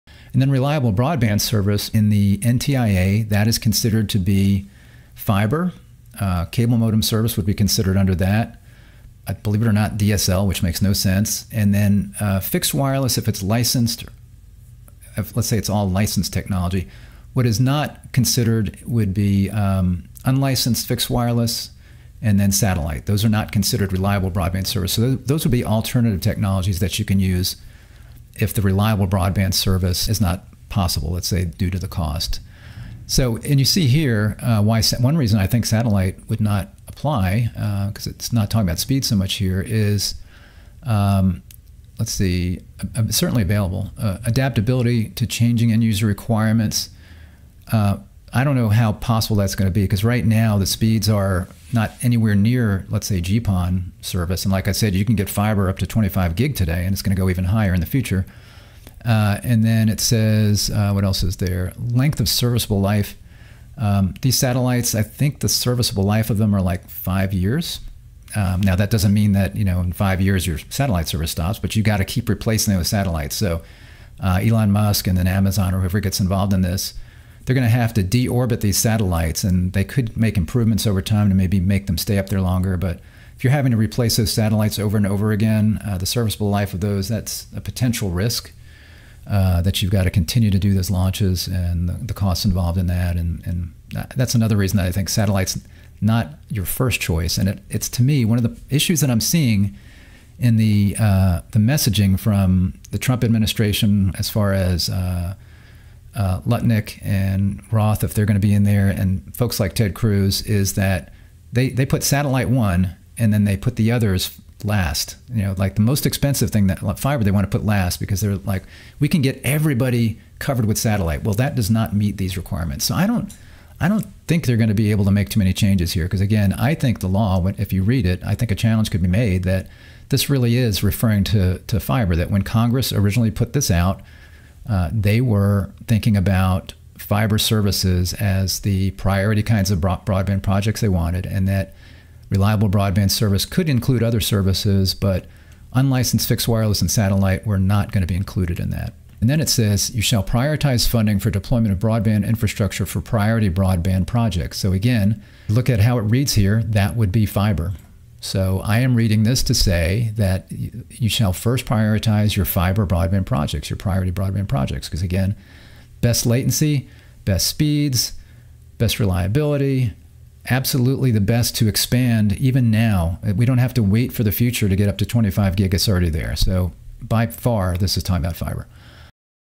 Is Satellite Reliable Broadband Service Under BEAD
There is a concern that the Commerce Department under the Trump Administration will require low earth orbit (LEO) satellite service like Starlink be funded in places where the states have already determined should get fiber. The Infrastructure Investment and Jobs (IIJA) specifically defines "Reliable Broadband" as "broadband service that meets performance criteria for service availability, adaptability to changing end-user requirements, length of serviceable life, or other criteria, other than upload and download speeds". It should be clear with a plain reading of this text that satellite service does not meet that requirement.